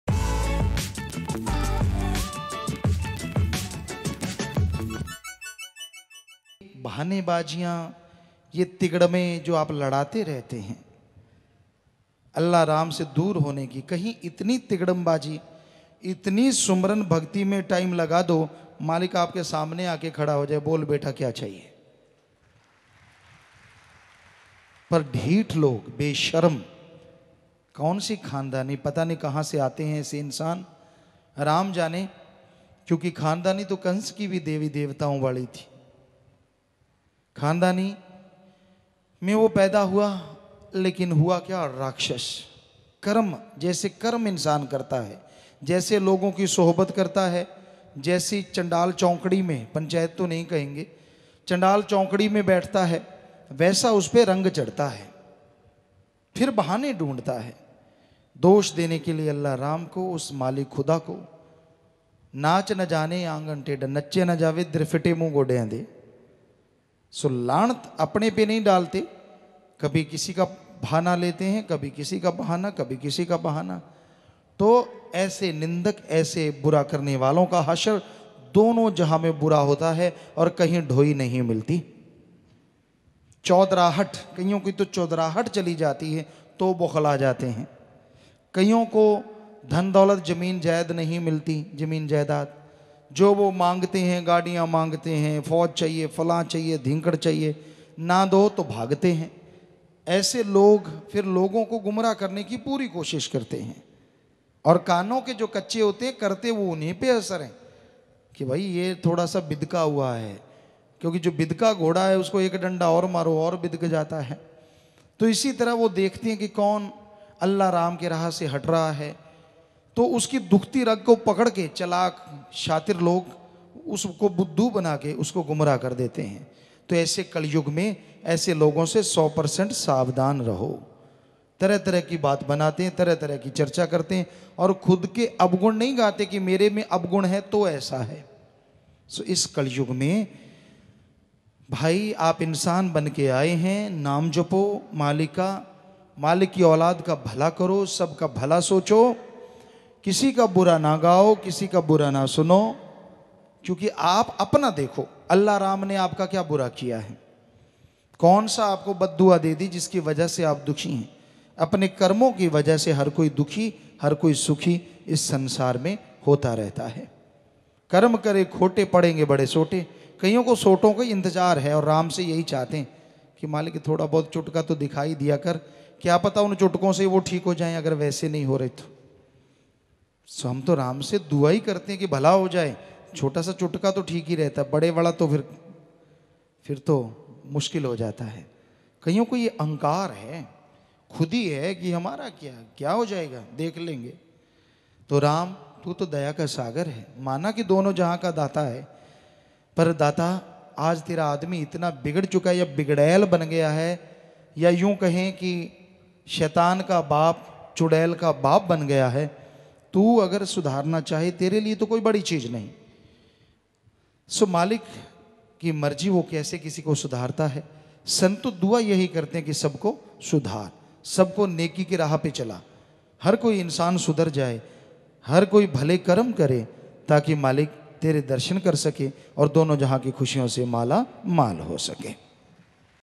निंदकों का हश्र दोनों जहाँ में बुरा ही होता है - Saint Dr. Gurmeet Ram Rahim Singh Ji Insan
निंदकों का हश्र दोनों जहाँ में बुरा ही होता है
The condition of slanderers remains worst in both worlds - Saint Dr. Gurmeet Ram Rahim Singh Ji Insan